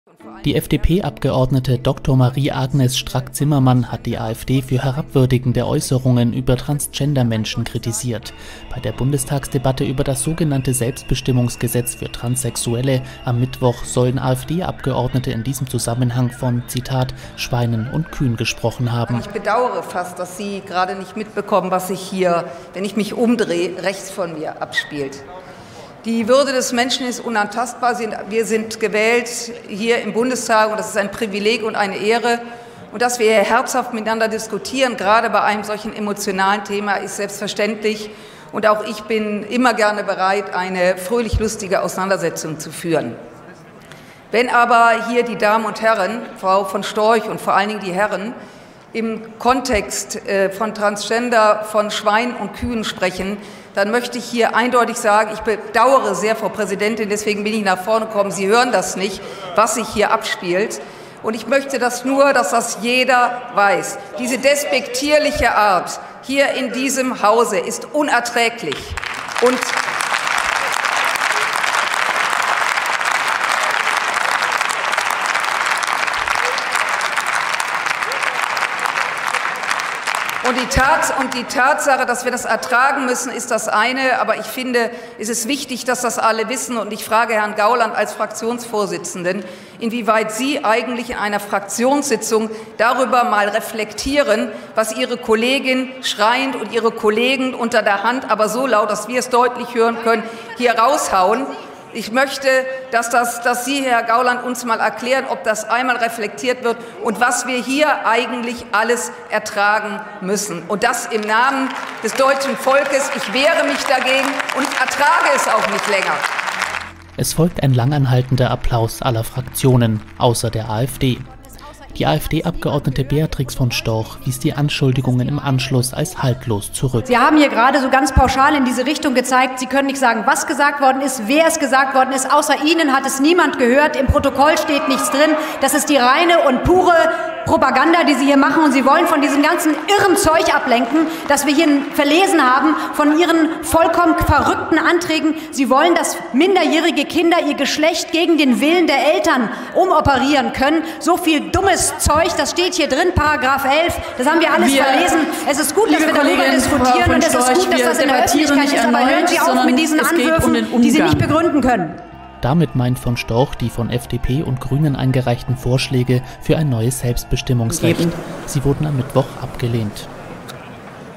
FDP-Frau trägt im Bundestag vor, was AfD-Abgeordnete während Debatte tuscheln
Die FDP-Abgeordnete Dr. Marie-Agnes Strack-Zimmermann hat die AfD für herabwürdigende Äußerungen über Transgender-Menschen kritisiert. Bei der Bundestags-Debatte über das sogenannte Selbstbestimmungsgesetz für Transsexuelle am Mittwoch sollen AfD-Abgeordnete in diesem Zusammenhang von „Schweinen und Kühen“ gesprochen haben.

 „Wenn aber hier die Damen und Herren, Frau von Storch und vor allem die Herren, im Kontext von Transgender von Schweinen und Kühen sprechen, dann möchte ich hier eindeutig sagen: ich bedaure es sehr, Frau Präsidentin, deswegen bin ich nach vorne gekommen. Sie hören das nicht, was sich hier abspielt, und ich möchte nur, dass das jeder weiß, diese despektierliche Art hier in diesem Hause ist unerträglich.“

Es folgt ein langanhaltender Applaus aller Fraktionen – außer der AfD. Die AfD-Abgeordnete Beatrix von Storch wies die Anschuldigungen im Anschluss als haltlos zurück. Es sei „reine Propaganda“. Strack-Zimmermann wolle nur von dem „irren Zeug ablenken“.

Damit meint von Storch die von FDP und Grünen eingereichten Vorschläge für ein neues Selbstbestimmungsrecht. Sie wurden am Mittwoch abgelehnt.